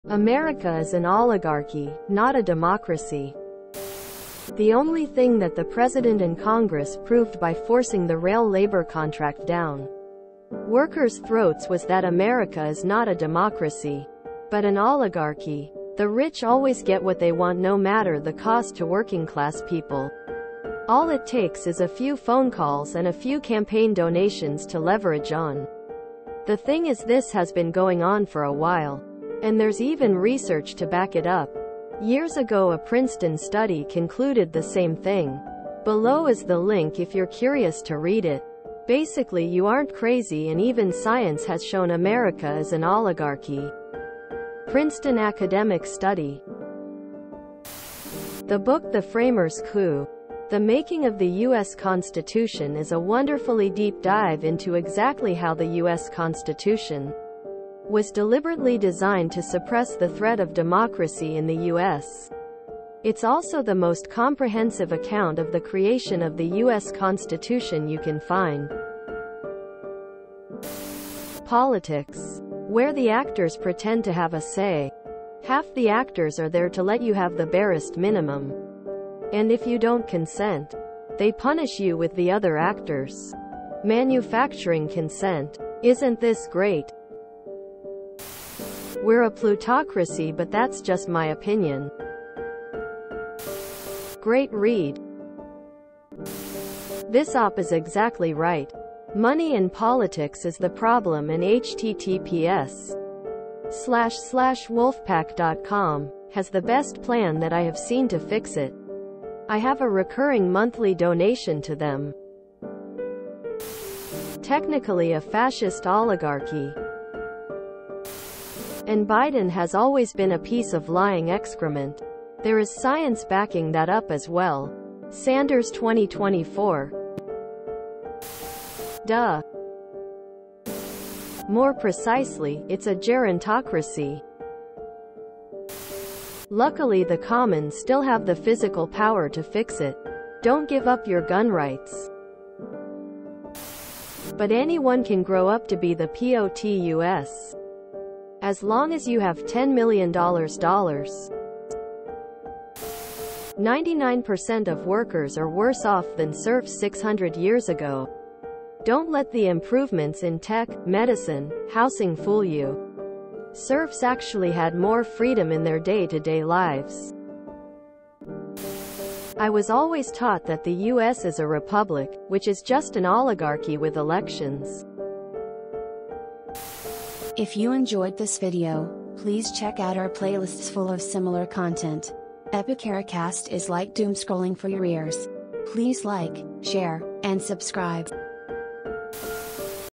America is an oligarchy, not a democracy
America is an oligarchy, not a democracy (r/antiwork)

Source: Dom2032 on Reddit (link removed at the insistence of YouTube)